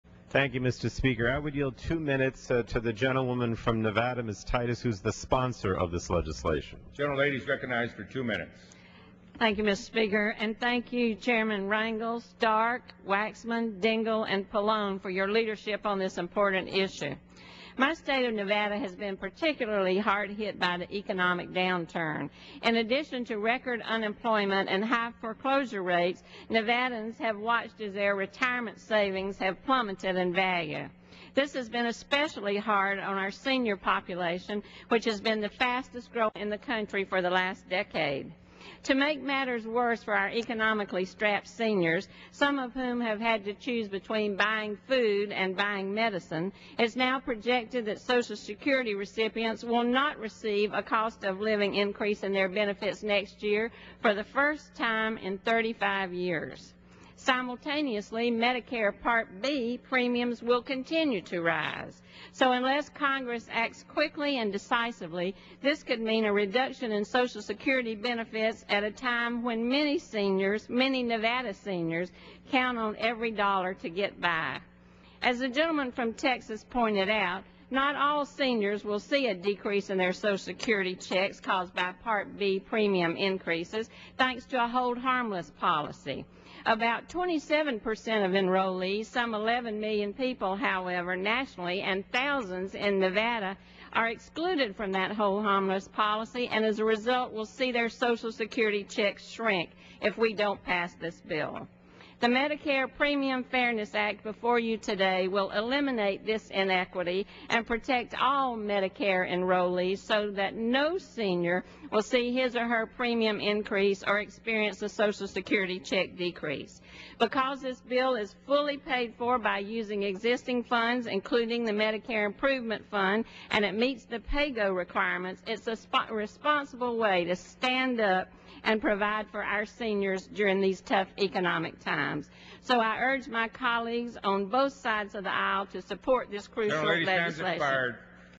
Titus Bill to Protect Seniors from Increase in Medicare Premiums 9/24/09
Congresswoman Titus speaks on the House floor on her bill to protect seniors from an increase in Medicare premiums.